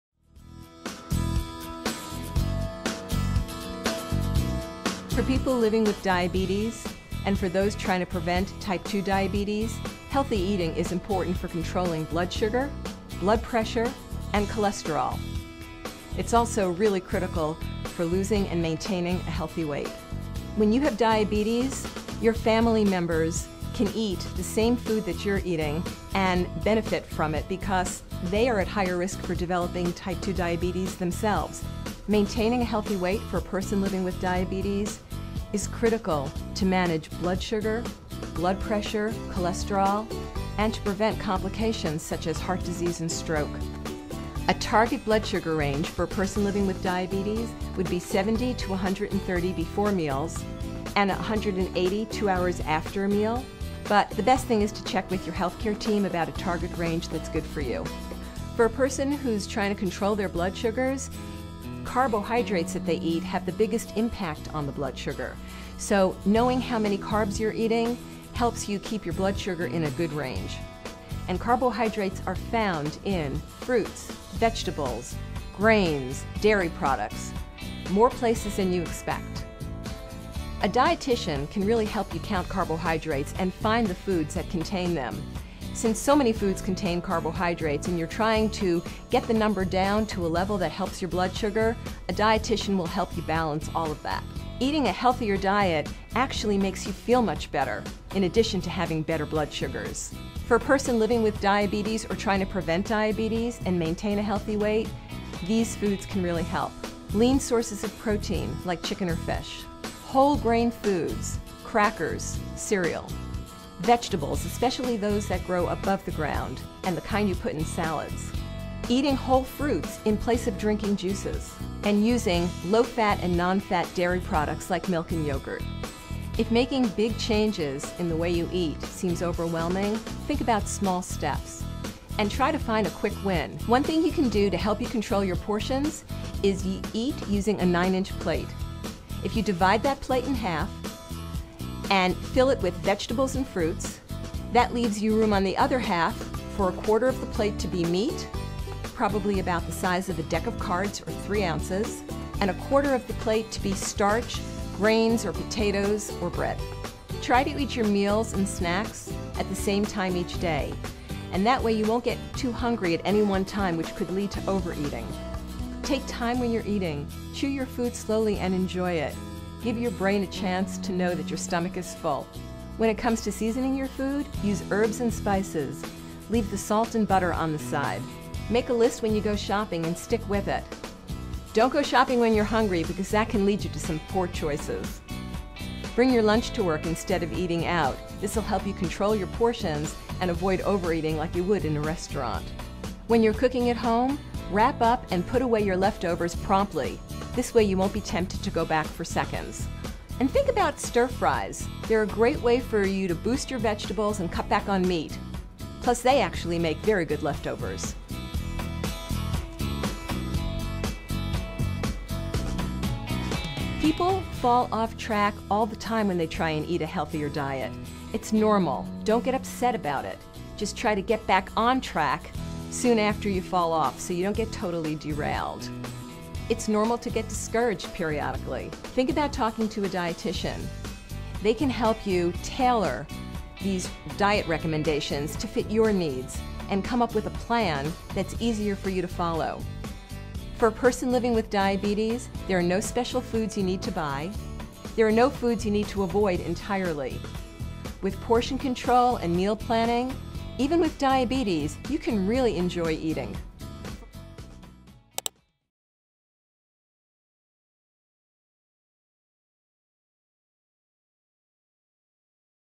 Healthy Eating with Diabetes
Making changes in the way you eat can be difficult. Learn about small steps for healthy eating to help you manage your weight.

Transcript:

For people living with diabetes and for those trying to prevent Type 2 Diabetes healthy eating is important for controlling blood sugar, blood pressure, and cholesterol. It's also really critical for losing and maintaining a healthy weight.

When you have diabetes, your family members can eat the same food that you're eating and benefit from it because they are at higher risk for developing Type 2 Diabetes themselves.

Maintaining a healthy weight for a person living with diabetes is critical to manage blood sugar, blood pressure, cholesterol, and to prevent complications such as heart disease and stroke. A target blood sugar range for a person living with diabetes would be 70 to 130 before meals and 180 two hours after a meal.

But the best thing is to check with your healthcare team about a target range that's good for
you. For a person who is trying to control their blood sugars, carbohydrates that they eat have the biggest impact on the blood sugar. So knowing how many carbs you're eating helps you keep your blood sugar in a good range. Carbohydrates are found in fruits, vegetables, grains, dairy products -- more places than you expect. A dietician can really help you count carbohydrates and find the foods that contain them.

Since so many foods contain carbohydrates and you're trying to get the number down to a level that helps your blood sugar, a dietician will help you balance all of that.

Eating a healthier diet actually makes you feel much better in addition to having better blood sugars. For a person living with diabetes or trying to prevent diabetes and maintain a healthy
weight, these foods can really help: lean sources of protein like chicken or fish; whole grain foods -- crackers, cereal; vegetables, especially those that grow above the ground and the kind you put in
salads; eating whole fruits in place of drinking juices and using low-fat and non-fat dairy products like milk and yogurt.

If making big changes in the way you eat seems overwhelming, think about small steps and try to find a quick win.

One thing you can do to help you control your portions is you eat using a nine-inch plate. If you divide that plate in half and fill it with vegetables and fruits that leaves you room on the other half for a quarter of the plate to be meat, probably about the size of a deck of cards or three ounces and a quarter of the plate to be starch, grains, or potatoes or bread. Try to eat your meals and snacks at the same time each day and that way you won't get too hungry at any one time which could lead to overeating.

Take time when you're eating. Chew your food slowly and enjoy it. Give your brain a chance to know that your stomach is full.

When it comes to seasoning your food use herbs and spices. Leave the salt and butter on the side.

Make a list when you go shopping and stick with it. Don't go shopping when you're hungry because
that can lead you to some poor choices. Bring your lunch to work instead of eating out. This will help you control your portions and avoid overeating like you would in a restaurant.

When you're cooking at home, wrap up and put away your leftovers promptly. This way you won't be tempted to go back for seconds. And think about stir-fries; they're a great way for you to boost your vegetables and cut back on meat, plus they actually make very good leftovers.

[music]

People fall off-track all the time when they try and eat a healthier diet. It's normal. Don't get upset about it. Just try to get back on-track soon after you fall off so you don't get totally derailed.

It's normal to get discouraged periodically. Think about talking to a dietician. They can help you tailor these diet recommendations to fit your needs and come up with a plan that's easier for you to follow. For a person living with diabetes there are no special foods you need to buy.

There are no foods you need to avoid entirely. With portion control and meal planning, even
with diabetes, you can really enjoy eating.

[music]